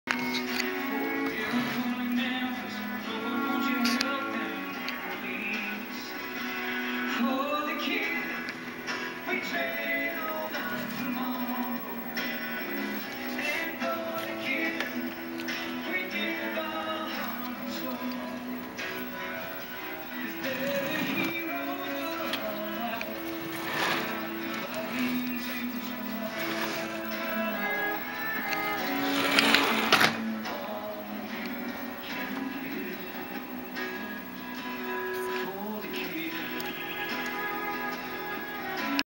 Lucas first time going outside on his own.